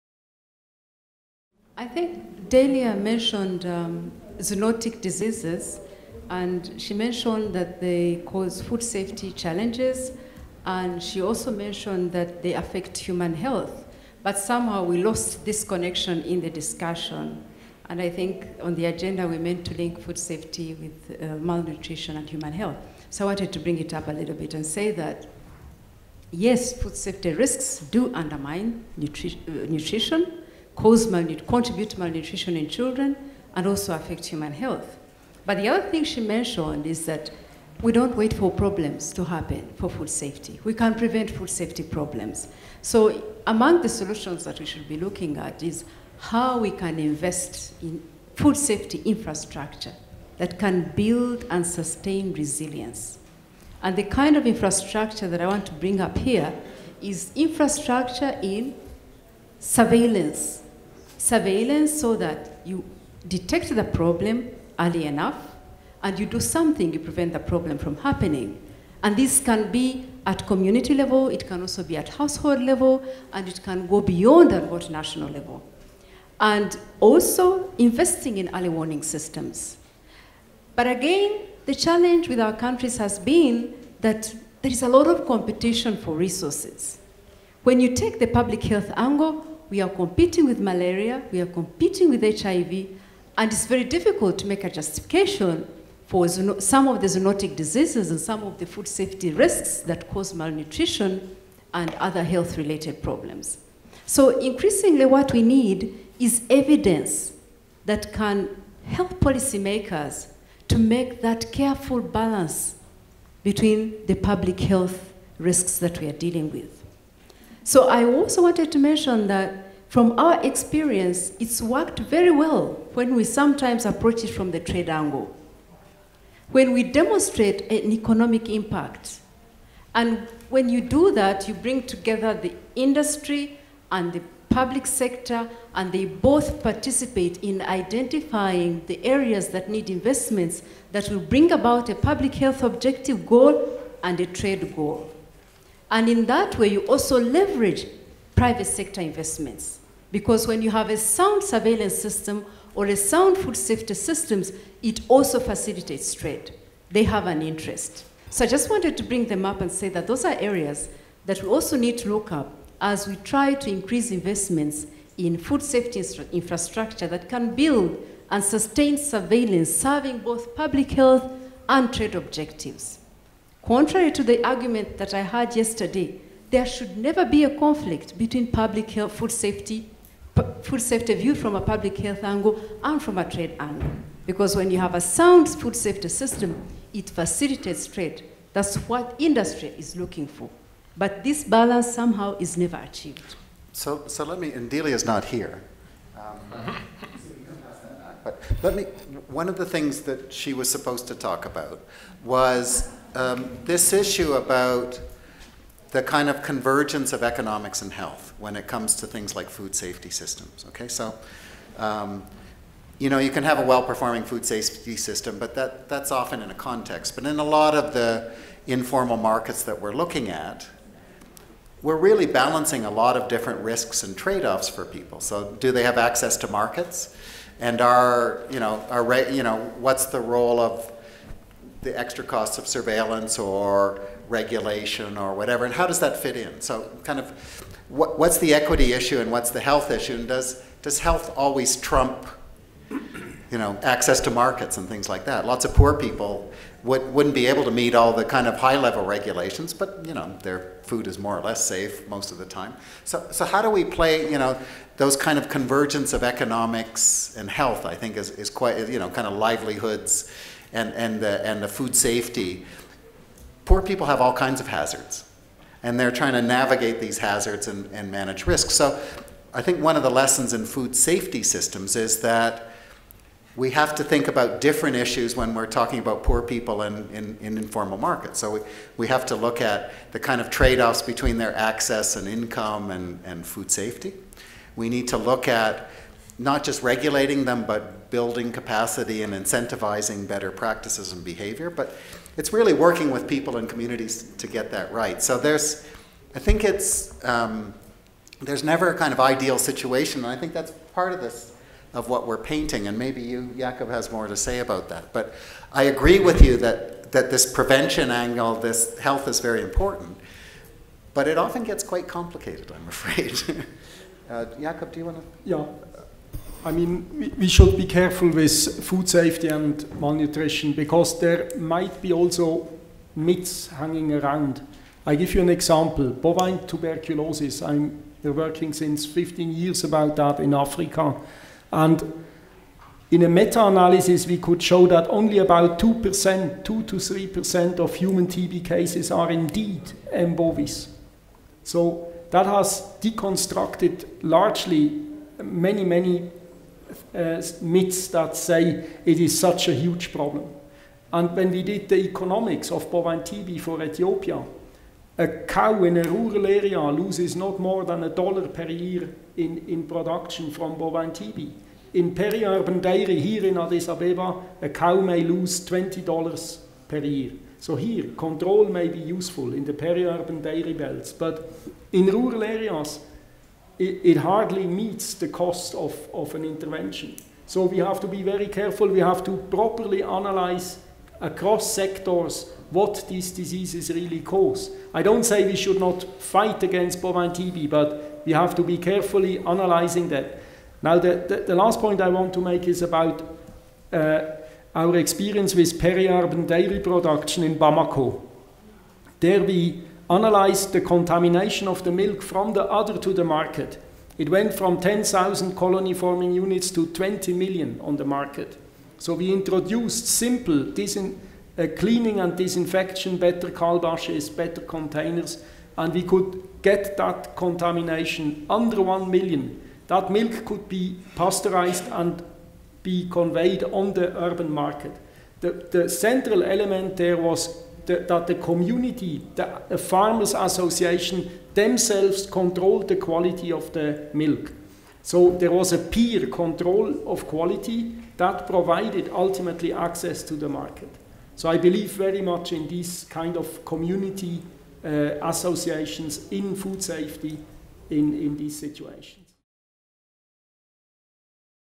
3F - Discussion Session (5) - Dealing with Food Safety, Nutrition, and Public Health Crises
Parallel Sessions 3: Dealing with Shocks. Discussion Session at Parallel Session 3F on "Health Shocks: Dealing with Food Safety, Nutrition, and Public Health Crises." IFPRI 2020 conference on Building Resilience for Food and Nutrition Security, May 15-17, 2014, Addis Ababa, Ethiopia.